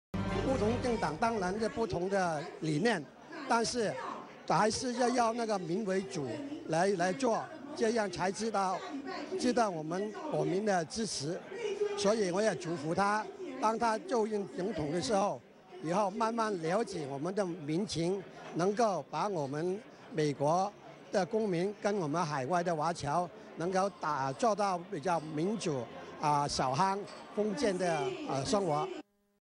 川普就職美夢成真
美國45任總統川普就職 羅省中華會館主席 方杰洲送祝福